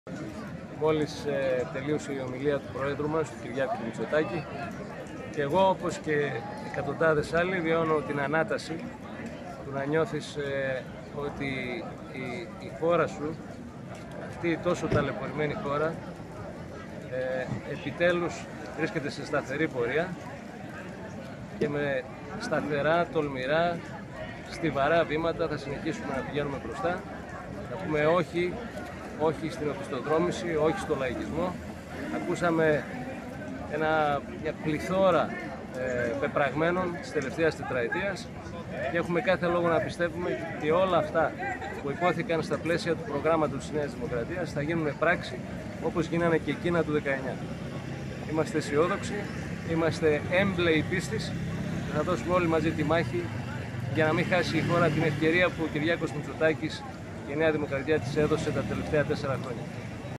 thrakionline.gr: Δ. Σαλτούρος για την ομιλία Μητσοτάκη στην Ξάνθη
thrakionline.gr: "Επιτέλους η ταλαιπωρημένη χώρα μας βρίσκεται σε σταθερή πορεία, δήλωσε μετα το τέλος της ομιλίας του Κυριάκου Μητσοτάκη ο υποψήφιος με την Νέα Δημοκρατία για τον Νομό Ξάνθης Δημήτρης Σαλτούρος.